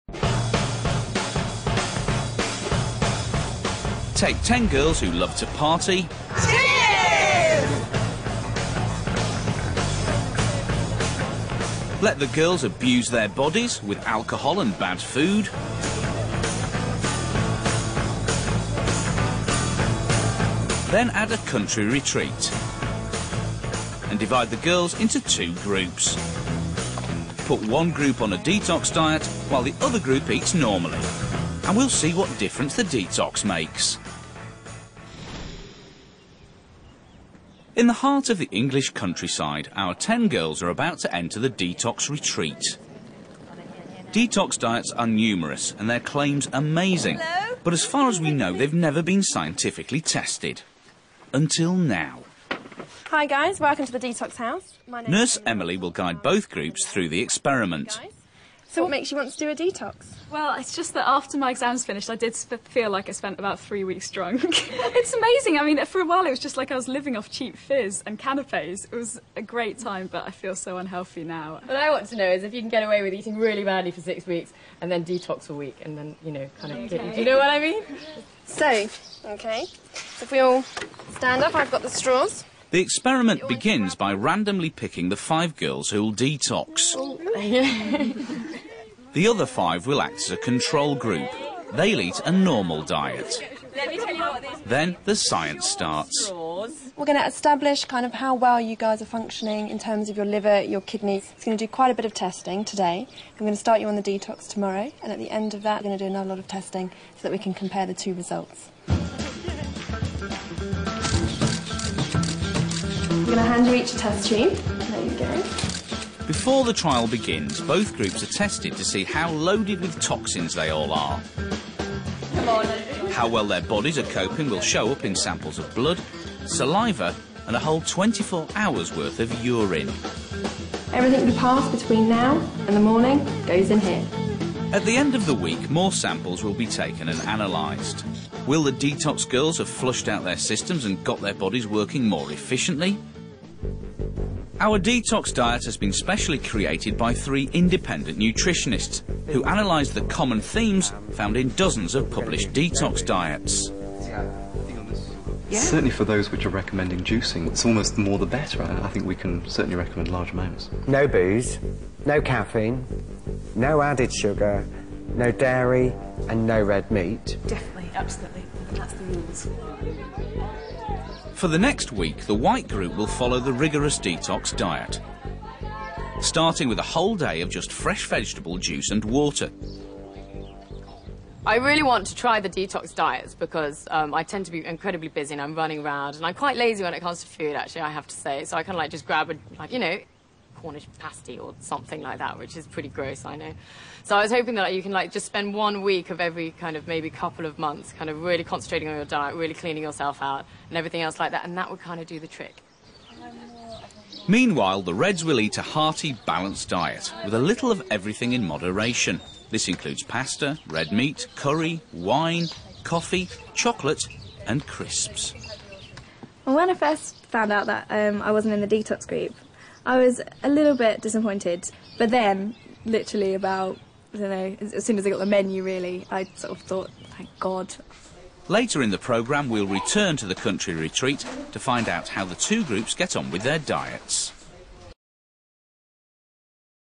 Party girls thrown in diet detox center - will it work? BBC
Ten girls are sent to a country retreat to find out if detoxing actually helps the body. Free dieting and weight loss programme video from BBC Worldwide.